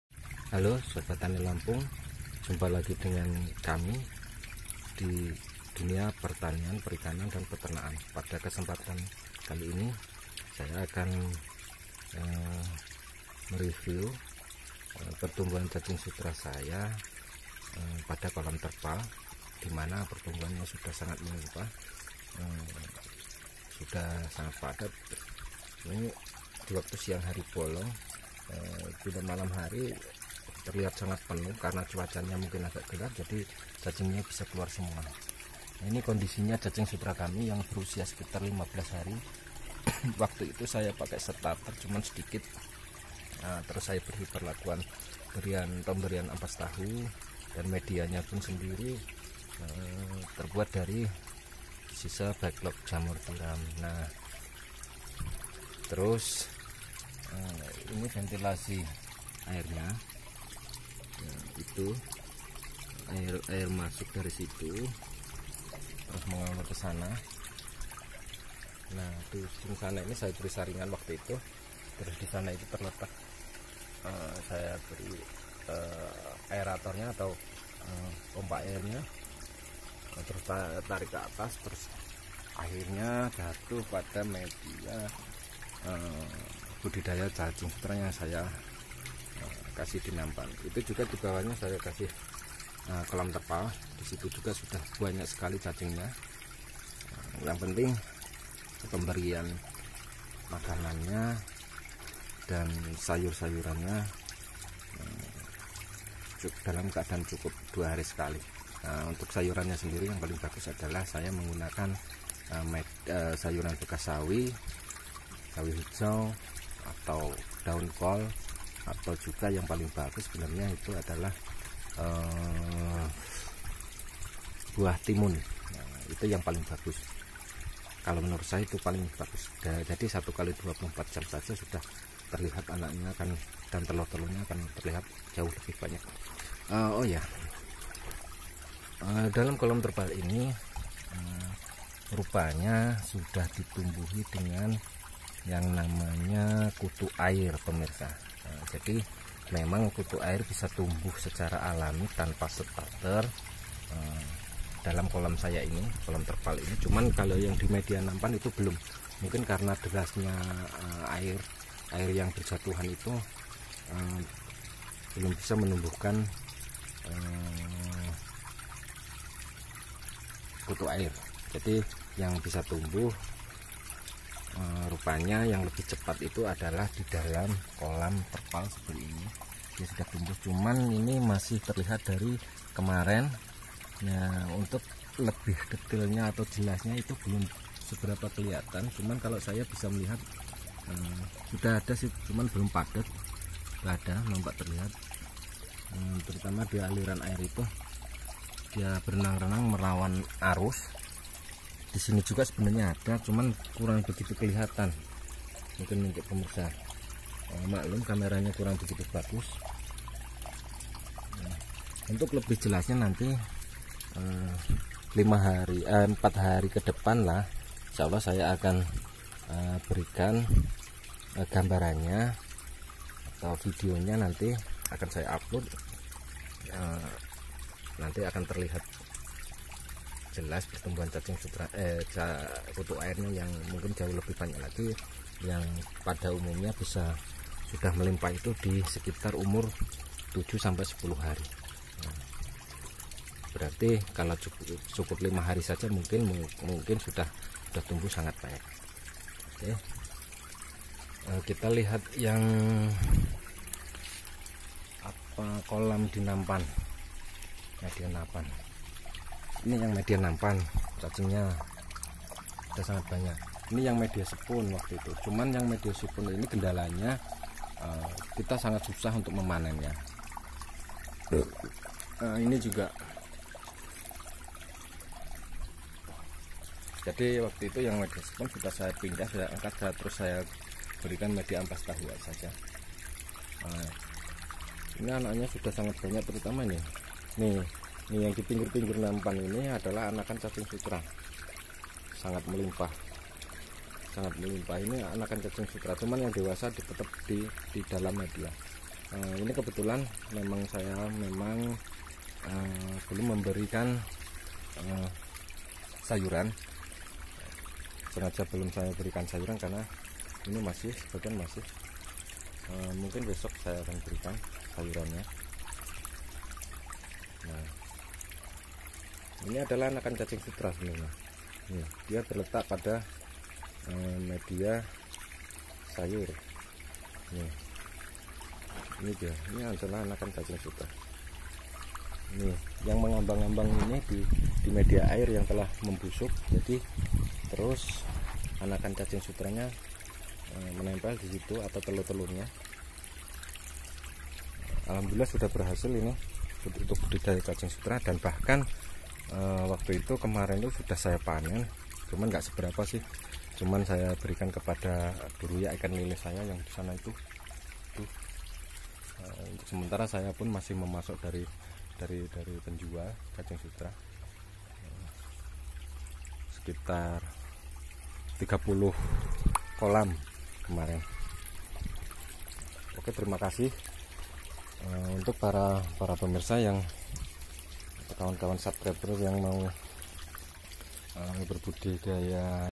budidaya cacing sutra & kutu air kolam terpal untuk pemula
cara mudah budidaya cacing sutra dan kutu air  kolam terpal mudah sederhana